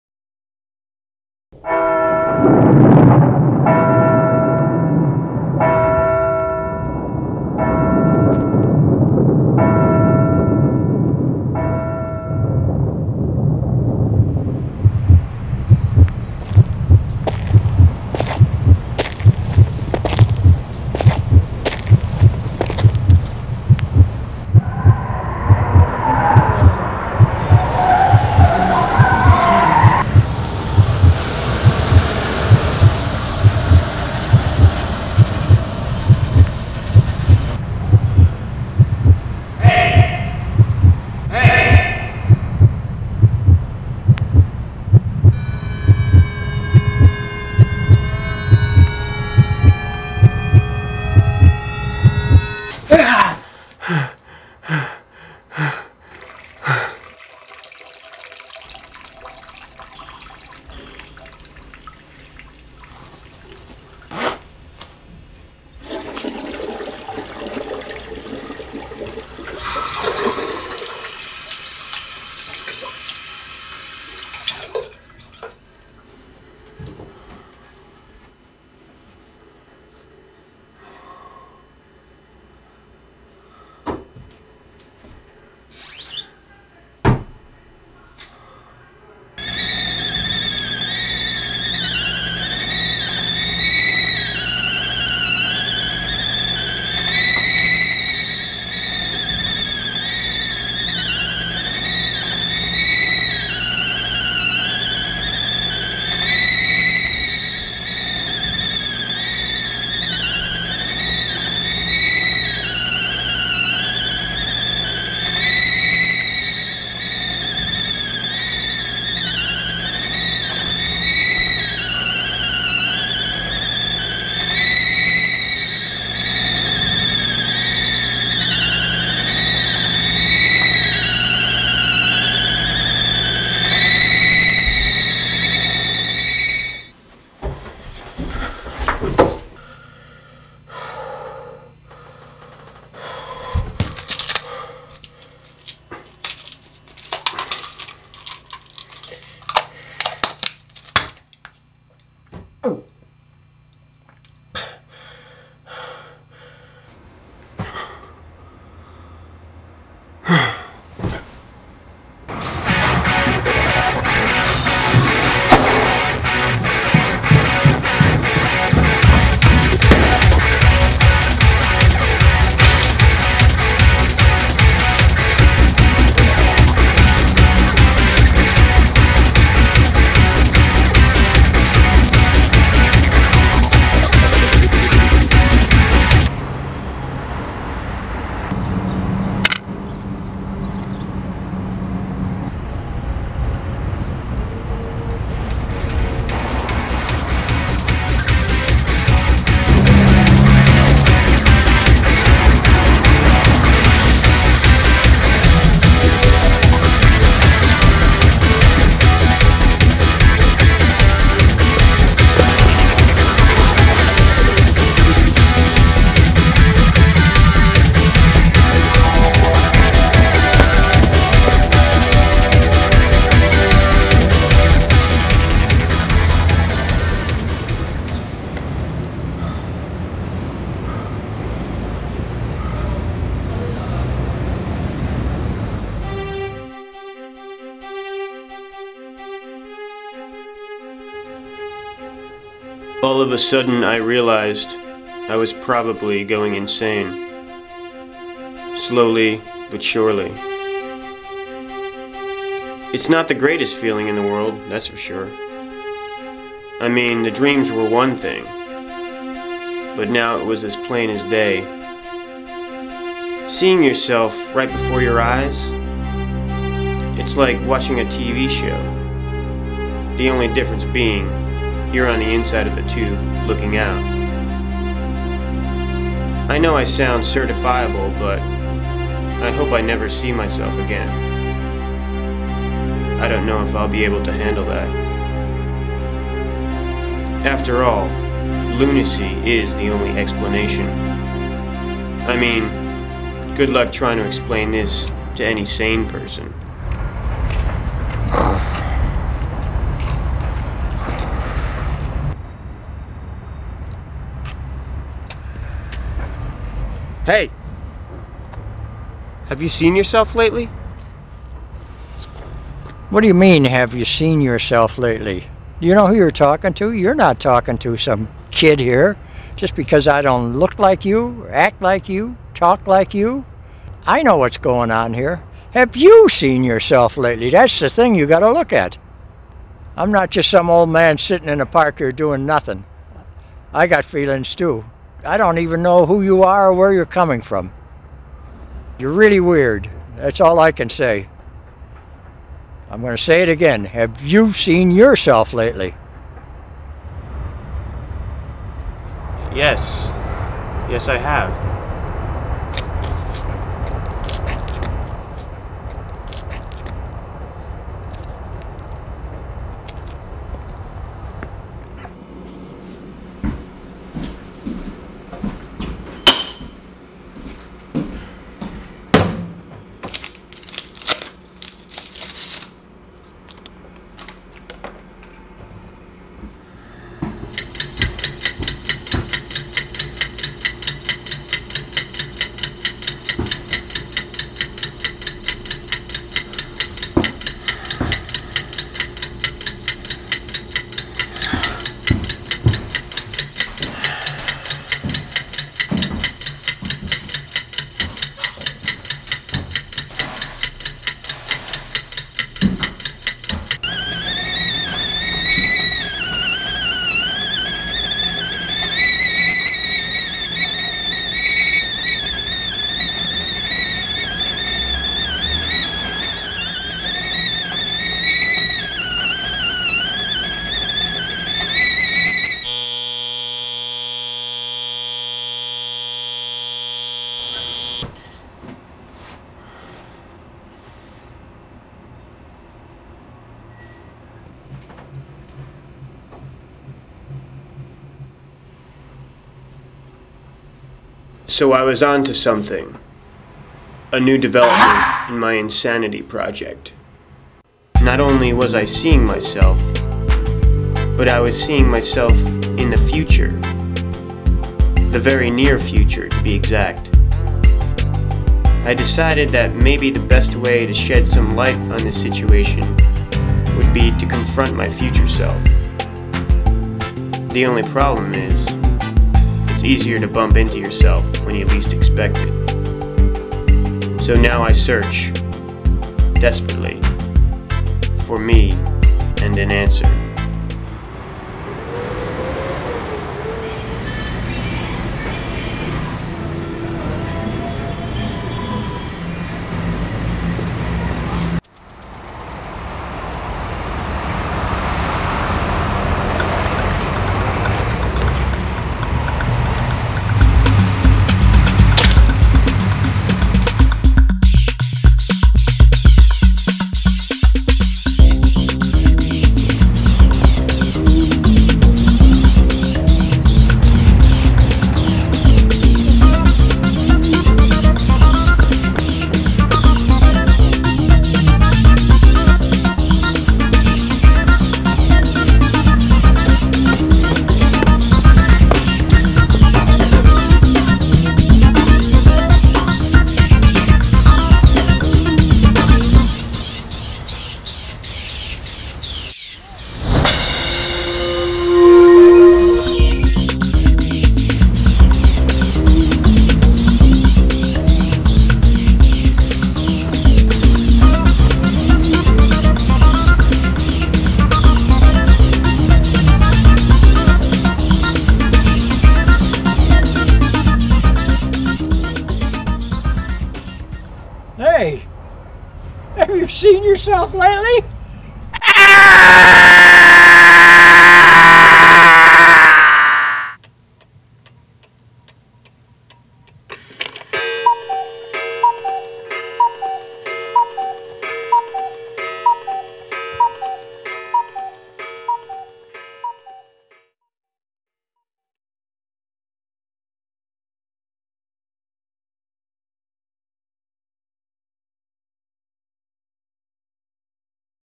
Double Time - Directed by David Palmieri
short film i did back in 2001.  shot on sony vx1000.  shot most of it by myself (eg. walking down hollywood blvd, CU of my face, using my right arm as a camera crane)  and the rest with one accomplice.  i was screwing around with some very simple but somewhat effective visual effects done in post:  split screens, some green screen, etc.